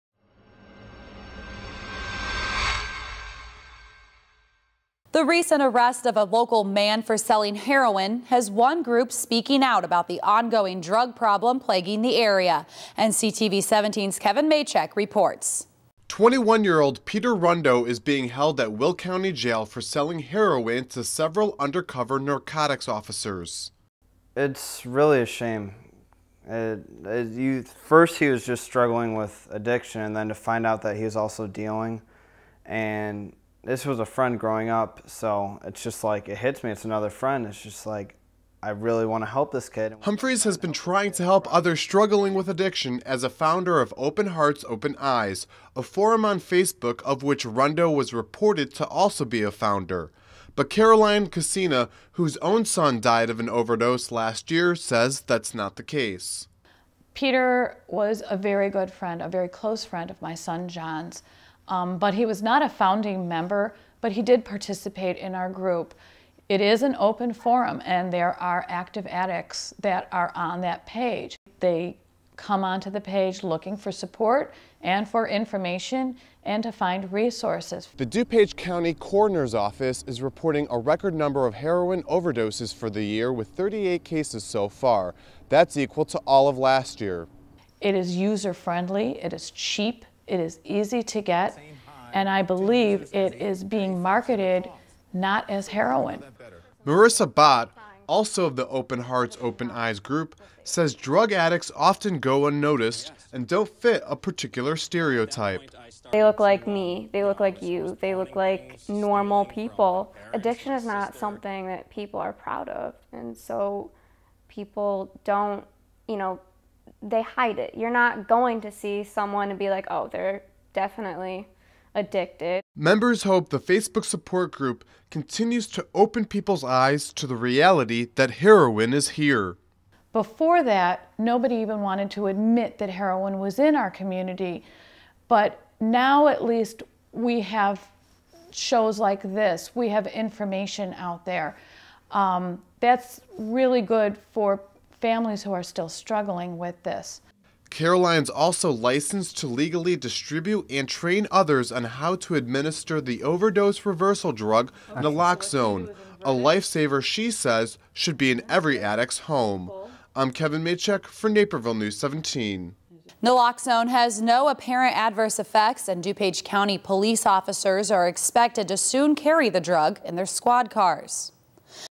Heroin-Related Arrest Draws More Attention to Drug Problem
21-year-old Peter Rundo is being held at Will County Jail for selling heroin to several undercover narcotics officers

"It's really a shame," said friend Jonathan Humphreys.  "At first he was just struggling with addiction and then to find out he was also dealing, and this was a friend growing up and it just hits me.  It's another friend and I really want to help this kid."

Humphreys has been trying to help others struggling with addiction as a founder of an Open Hearts Open Eyes, a forum on Facebook, of which Rundo was reported to be also be a founder.

But Caroline Kacena, who's own son died of an overdose last year, says that's not the case.

"Peter was a good friend of my son John but he was not a founding member but he did participate in the group," she said.  "It is an open forum and there are active acts on that page.  They come there seeking information and to find resources."

The DuPage County Coroner's Office is reporting a record number of heroin overdoses for the year, with 38 cases so far.  That's equal to all of last year.

"It is cheap.  It is user friendly, easy to get and I believe it is being marketed as not as heroin," said Kacena.  "I've talked to several families and young people who've been given heroin unknowingly, as my son was, and it's being marketed as other substances."

Marissa Bahtt, also of the open hearts, open eyes group, says drug addicts often go un-noticed and don't fit a particular stereotype.

"They look like you.  They look like me," said Bahtt.  "Addiction is something people are not proud of so they try to hide.  It you're not going to see someone and be like, oh they're definitely an addict."

Members hope the Facebook support group continues to open people's eyes to the reality that heroin is here.

"Before that, nobody wanted to even admit that heroin was in our community," said Kacena.  "Now we at least have shows like this.  We have information out there for families still struggling with this."

She's also licensed to legally distribute and train others on how to administer the overdose reversal drug Naloxone, a life-saver she says should be in every addict's home.

Naloxone has no apparent adverse effects and DuPage County police officers are expected to soon carry the drug in their squad cars.